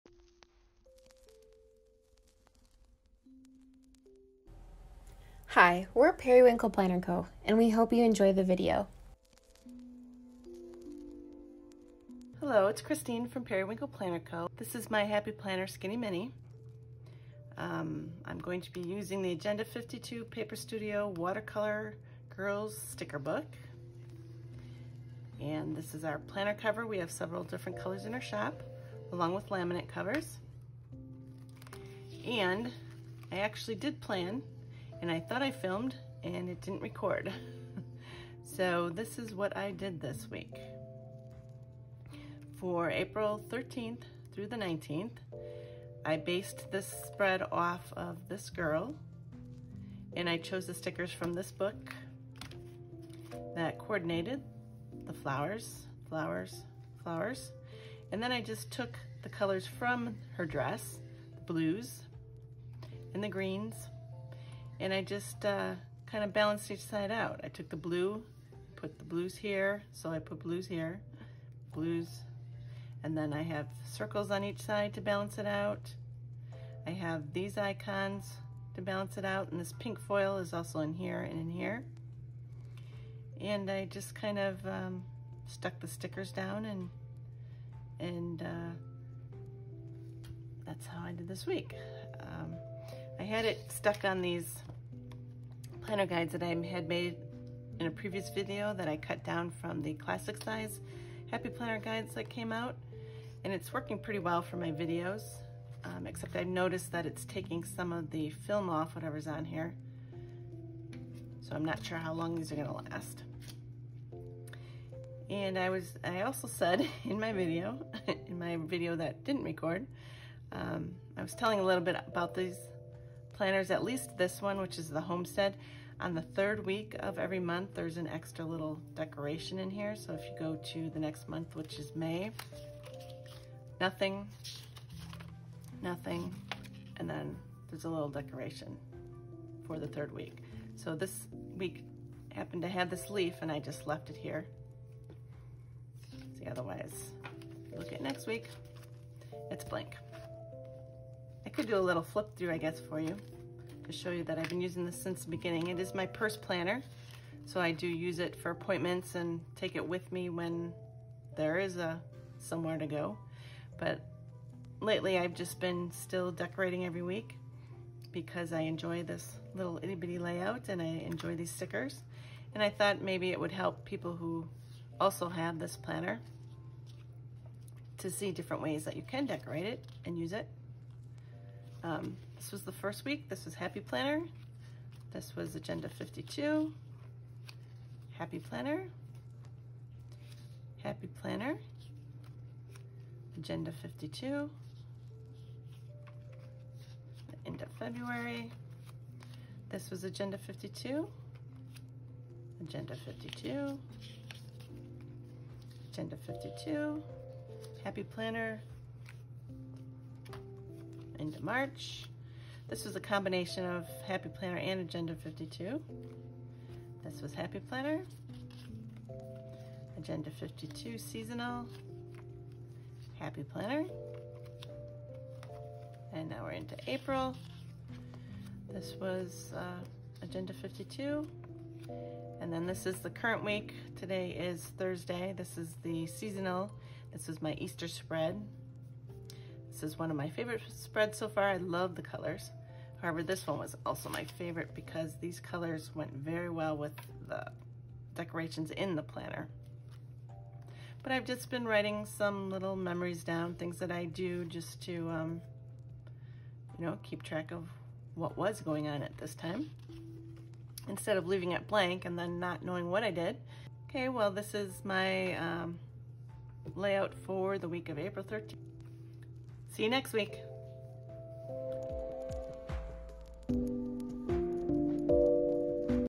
SKINNY mini Happy Planner | Plan With Me | Agenda 52 | The Paper Studio Stickers
I LOVE using The Planner Studio Agenda 52 stickers in my Skinny Mini Happy Planner.  The fit is perfect.